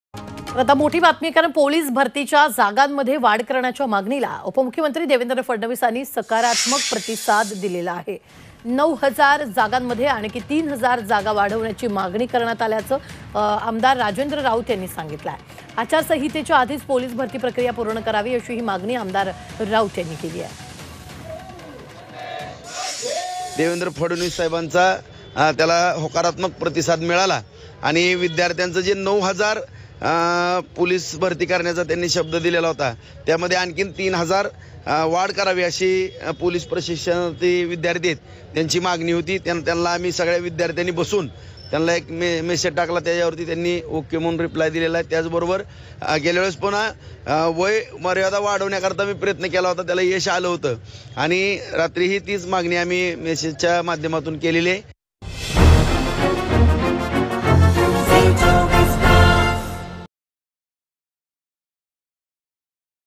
आचारसंहितेपूर्वी पोलीस भरती जाहिरात निघणार...12,000 पदांची पोलीस भरती जाहिरात लवकरच...👮‍♂️🚔
आचारसंहितेपूर्वी पोलीस भरती जाहिरात निघणार...

12,000 पदांची पोलीस भरती जाहिरात लवकरच...👮‍♂️🚔✌️